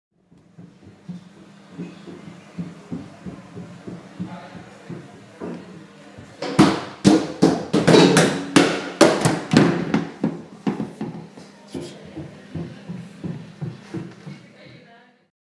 Streamlight prueba caida
Prueba de resistencia caída linterna Streamlight